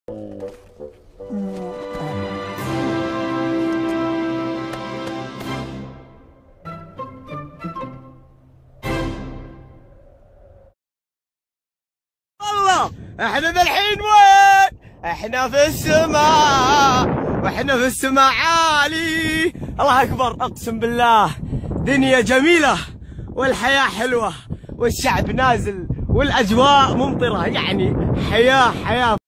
جندي يشعل مواقع التواصل بمقطع كوميدي أثناء إسقاطه باليمن
يشعل جندي يشعل مواقع التواصل بمقطع كوميدي أثناء إسقاطه باليمن